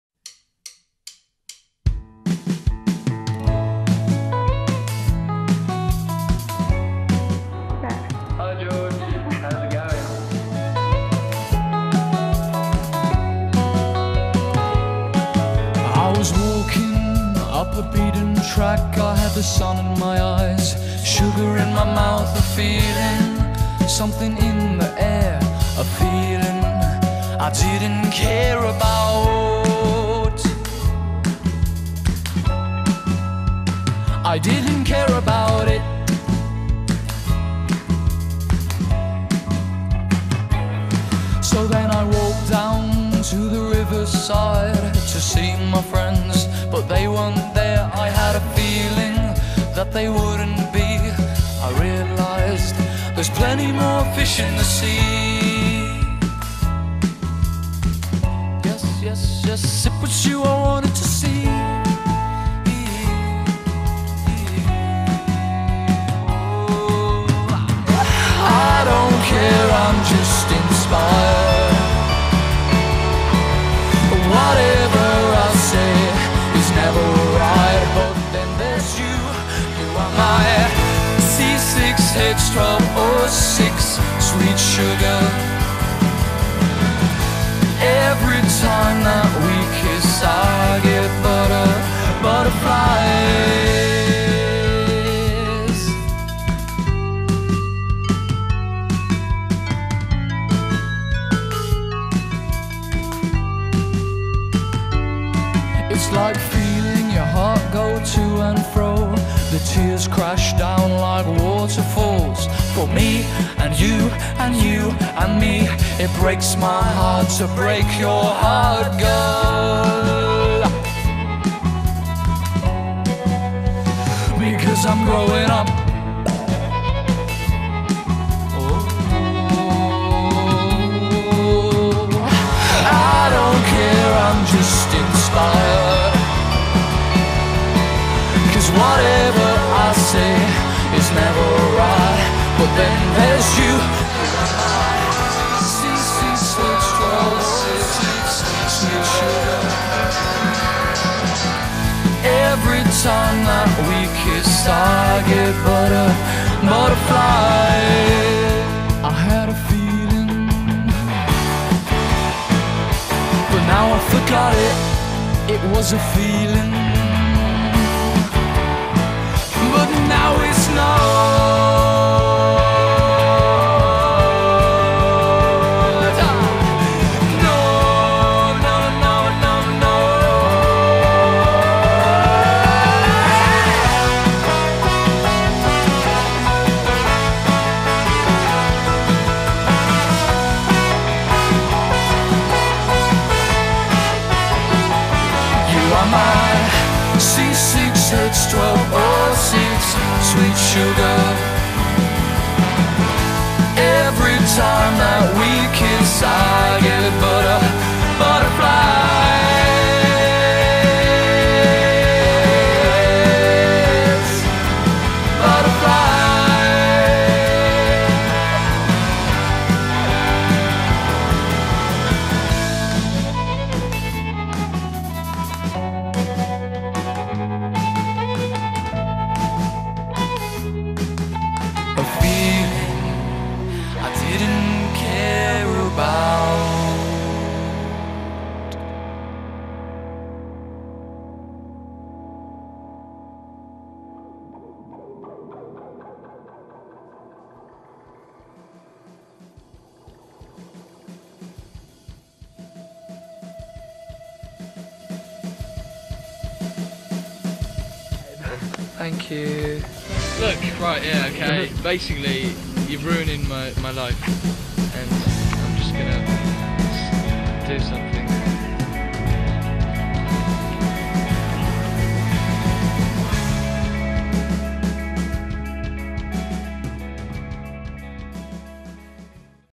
George Craig - One Night Only
George craig One night only, all the videos in one , just him...

Recopilado de todos los videos que andan dando vuelta por internet

Heermoso